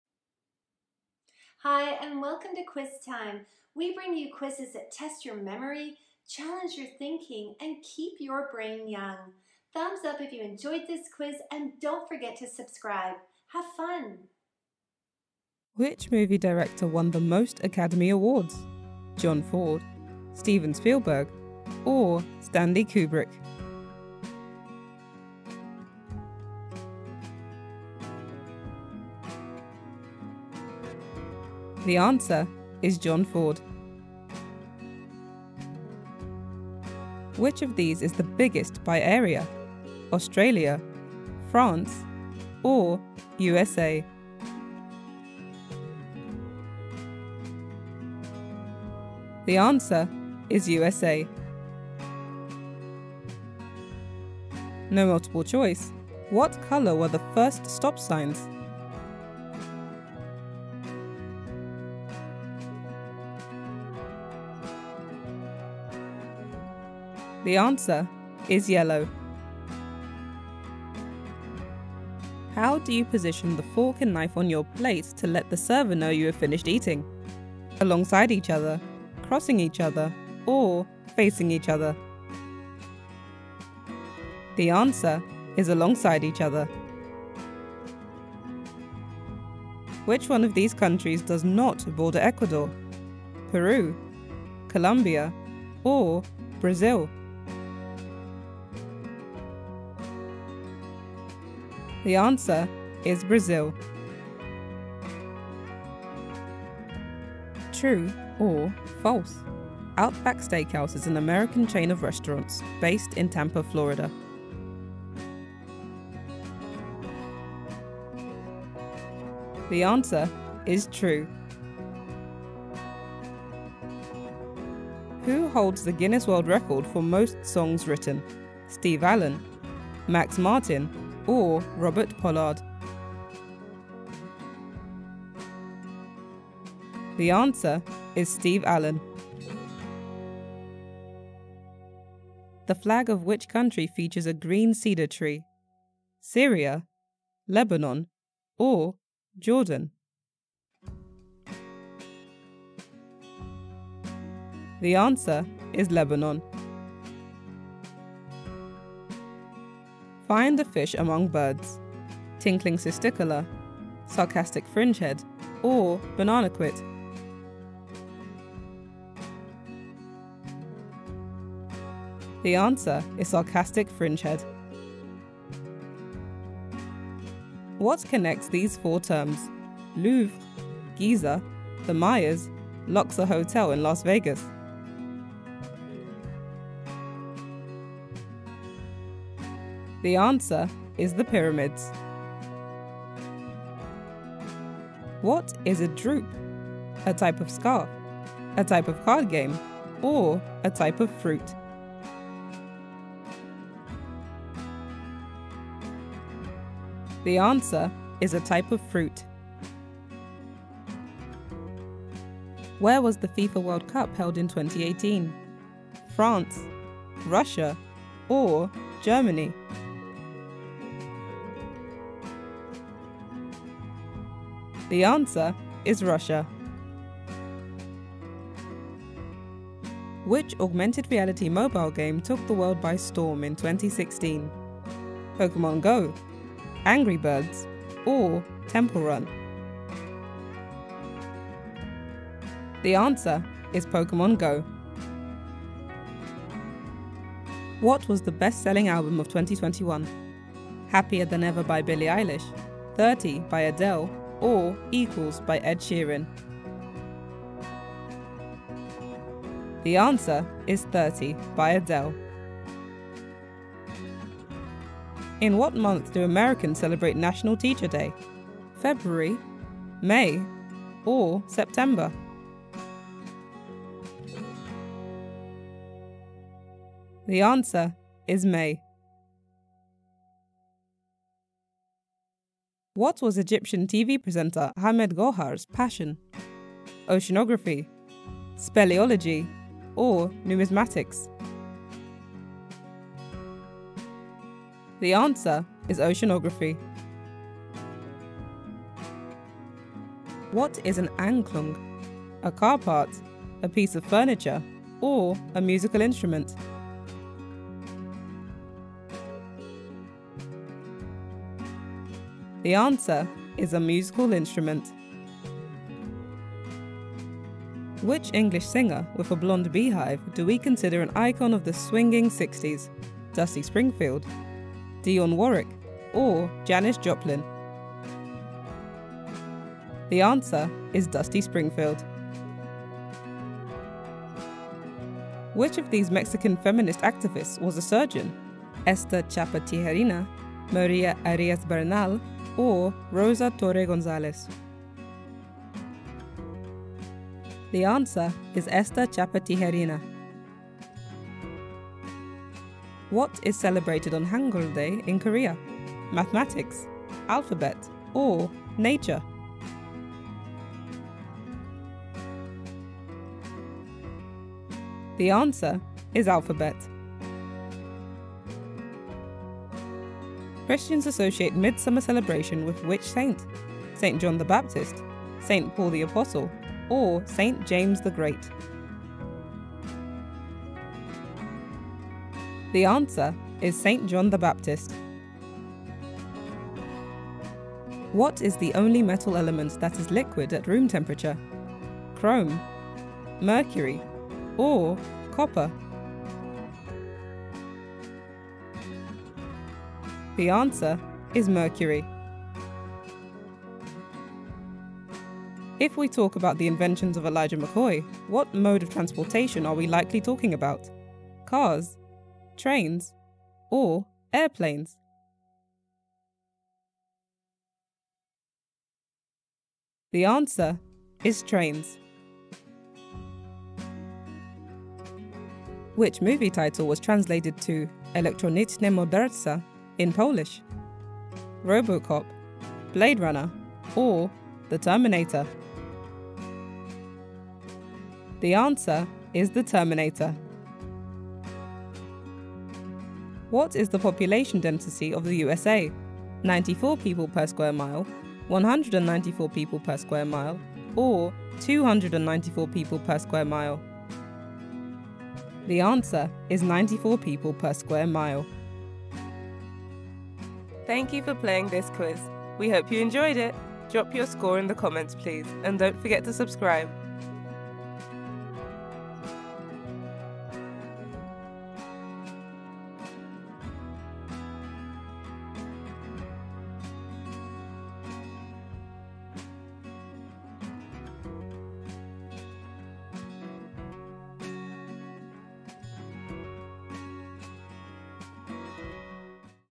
Can You Think? Try This Tough Trivia Quiz!
Let's see how far you can come in this tough trivia quiz! 25 questions and answers will appear. How many can you answer? Keep your score and let me know your score in the comments please! Have fun!

P.S.: we do our best to make good quizzes, do you see something wrong, a mispronunciation or a misspelling? We are sorry for that, but these are for fun. Don't get upset about it please.